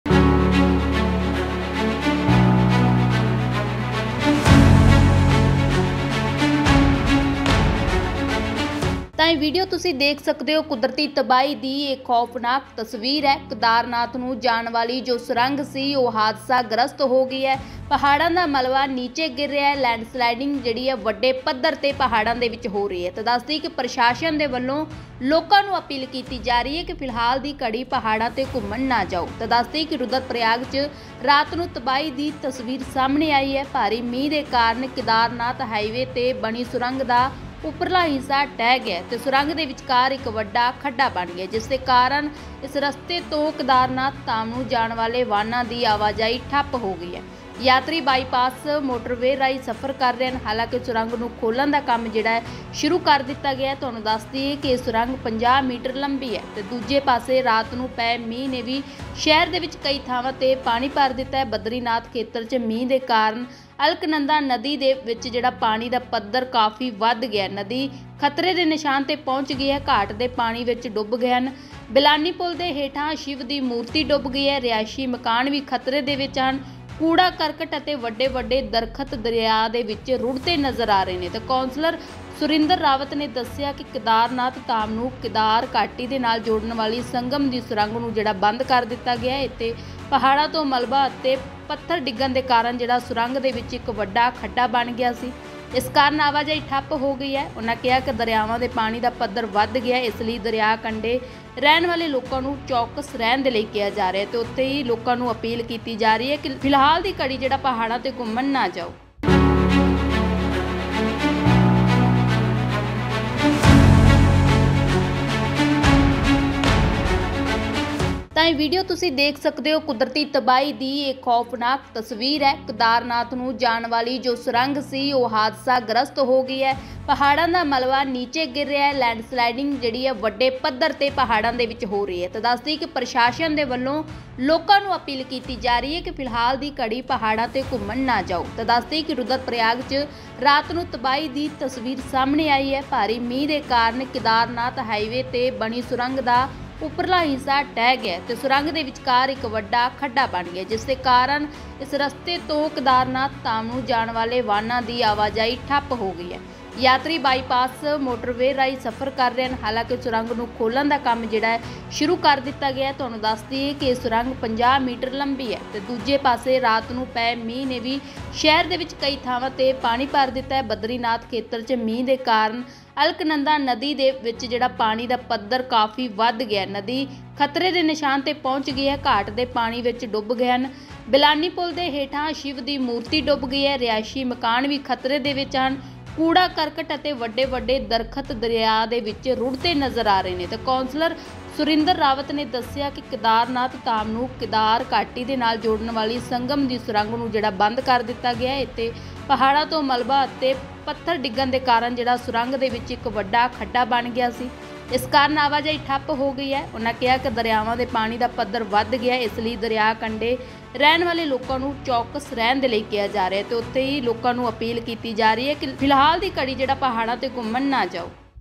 ਕੇਦਾਰਨਾਥ ਨੂੰ ਜਾਣ ਵਾਲੀ ਸੁਰੰਗ ਦਾ ਹੋਇਆ ਬੁਰਾ
ਲੈਡਸਲਾਈਡਿੰਗ ਹੋਣ ਕਾਰਨ ਡਿੱਗੇ ਵੱਡੇ-ਵੱਡੇ ਪੱਥਰ